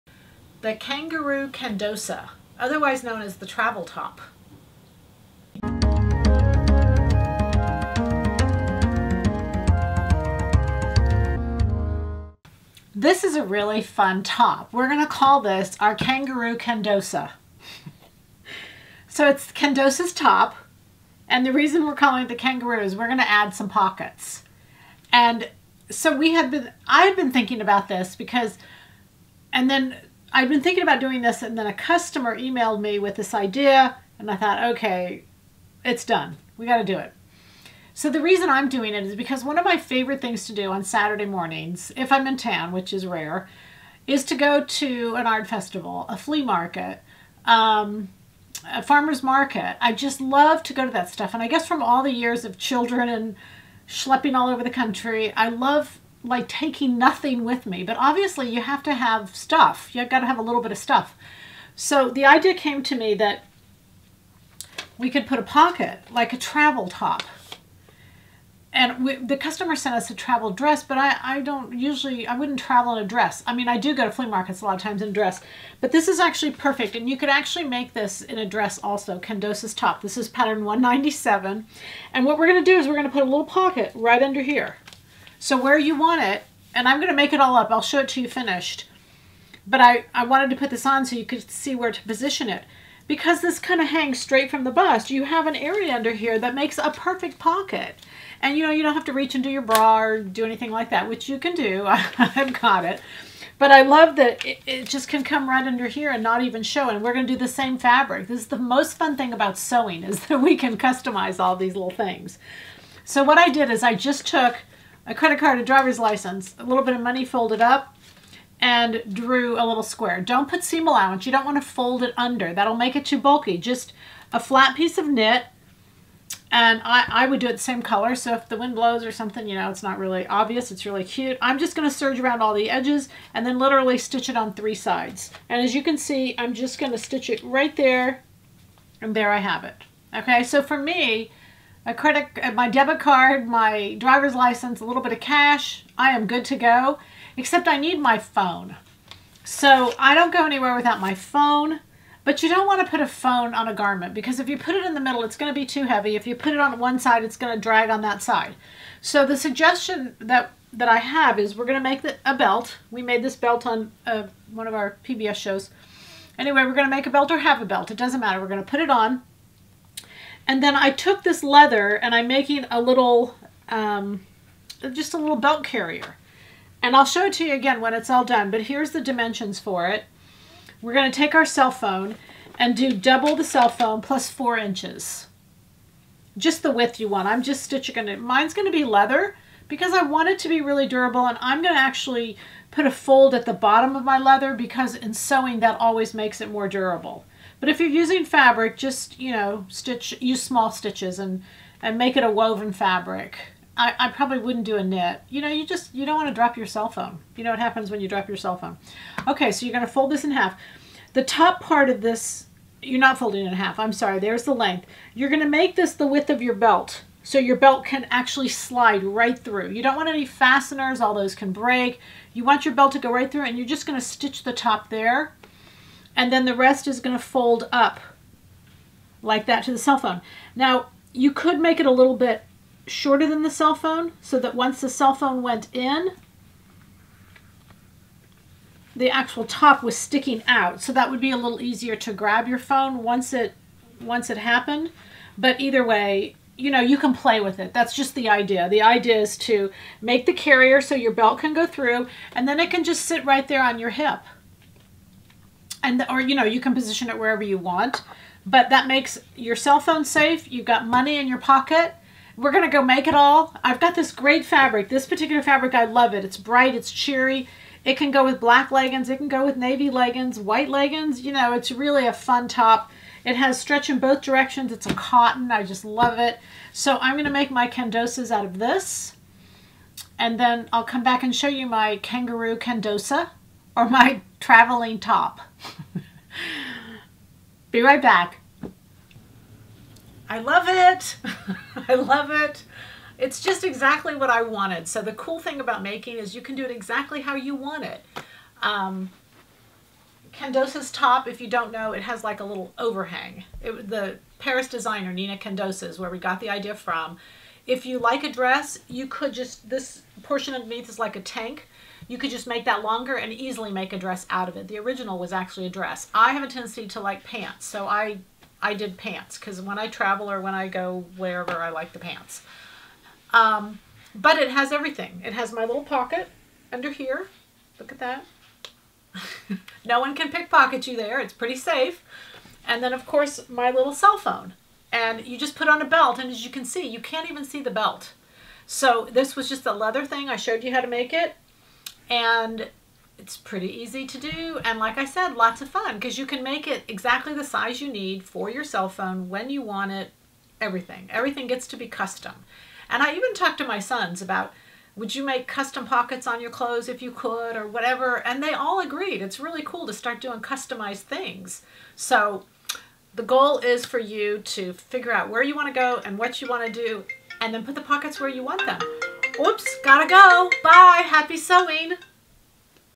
Let's Sew - The Travel Top - Episode 37
This episode covers a great travel top, with style options and a concealed pocket to keep your valuables safe.

You don't need a zipper on your Kangaroo pocket, just don't do any cartwheels without one.